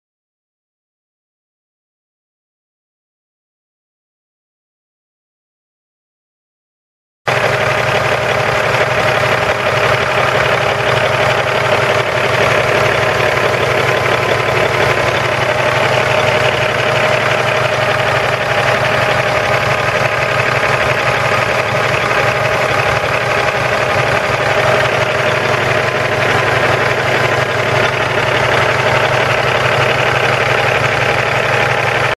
2004 International 9400i SBA semi truck for sale | no-reserve Internet auction January 12, 2017
2004 International 9400i SBA semi truck
1,060,741 miles on odometer
Cummins ISX 14.9L L6 turbo diesel engine
435 HP
Serial 6001801
Meritor 10 speed manual transmission
Tandem axle
Tilt
Telescoping steering wheel
Cruise
60" sleeper
Single bed
Ether start
Exterior visor
Air suspension
Air brakes
234" wheelbase
295/75R22.5 tires
GVWR 52,000 lbs
Kansas title
3HSCNAPR84N086073
It's located in Mentor, KS, and we're selling it to the highest bidder regardless of price.